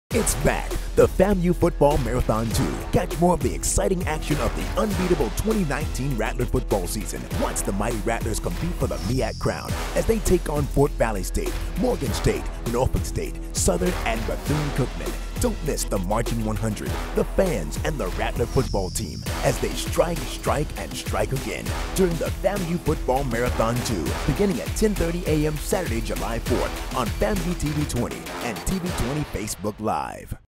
FAMU Football Marathon
Enjoy the holiday weekend, Rattler style! FAMU Athletics is hosting the second FAMU Football Marathon this Saturday, July 4 from 10:30 a.m. - midnight.

Support the Rattler Athletic Fund at: my.famu.edu/giving/marathon-2.